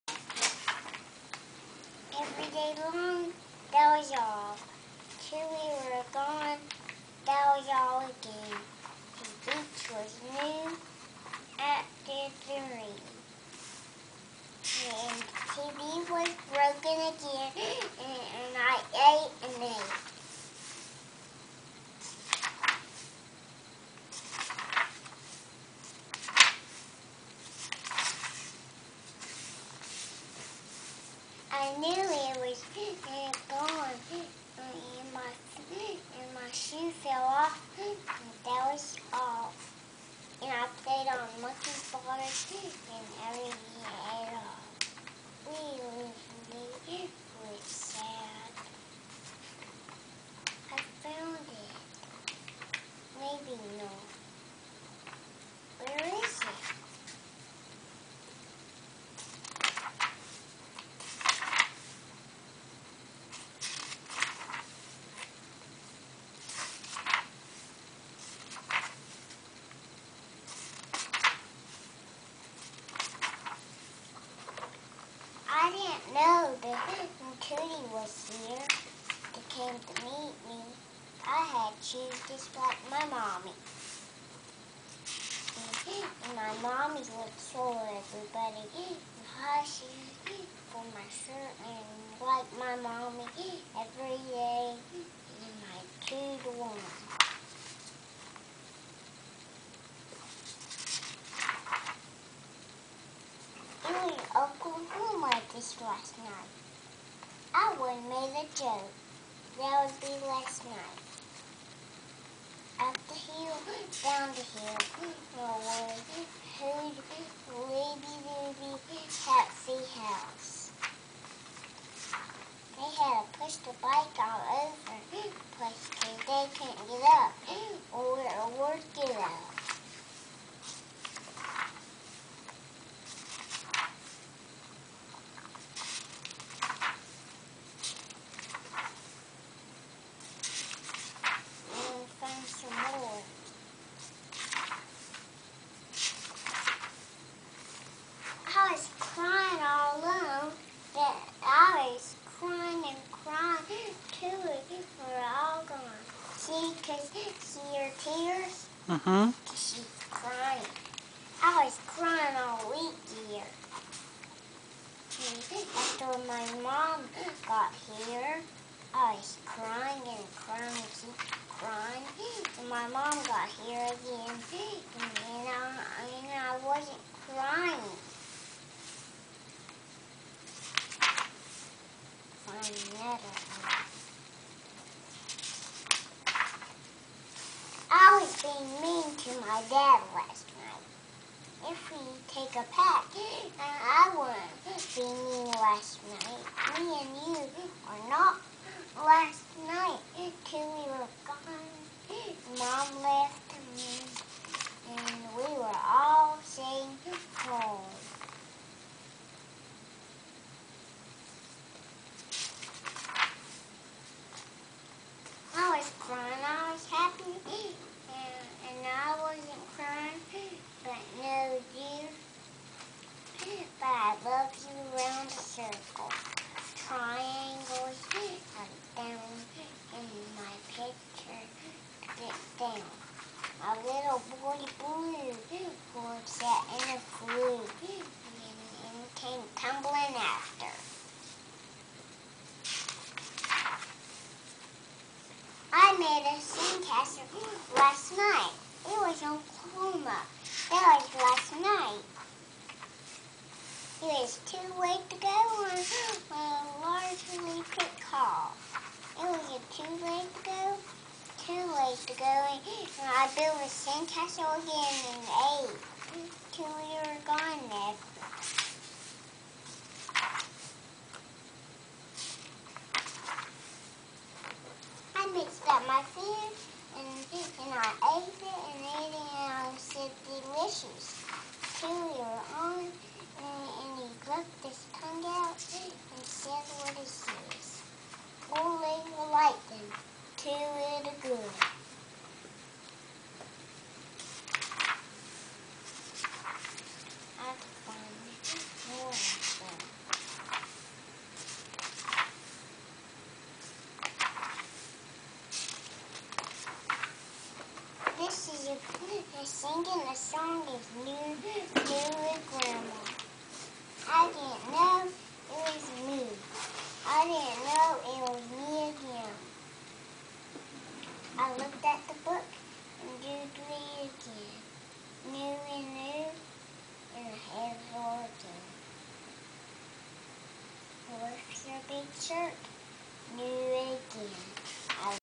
Megan is reading "Positive Discipline
Megan reading a book about discipline - 5/27/08.